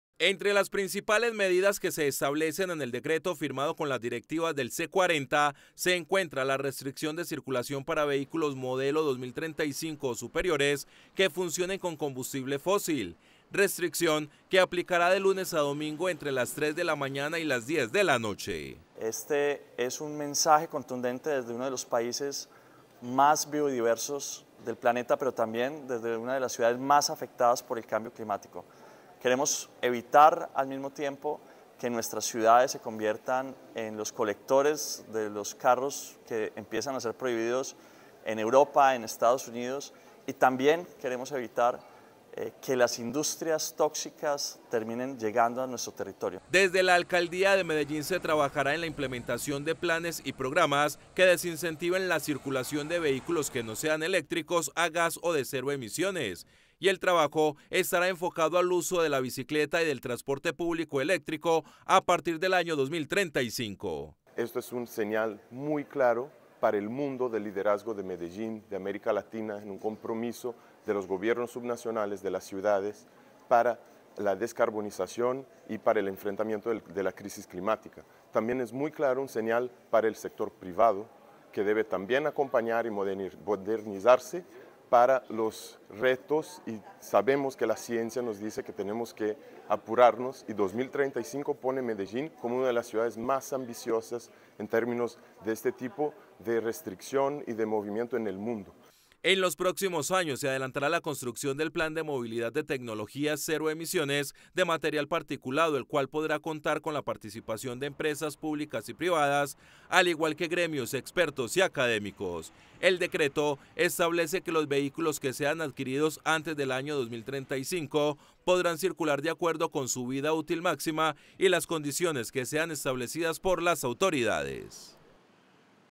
Listo decreto que restringe vehículos con combustible fósil en Medellín desde 2035 - Telemedellín
NoticiasTM NTM Desde la cumbre de gobernantes del c40 que se adelanta en Buenos Aires, Argentina, el alcalde de Medellín, Daniel Quintero, firmó el decreto con los mecanismos de movilidad sostenible e inteligente para el distrito con vigencia a partir el 2035.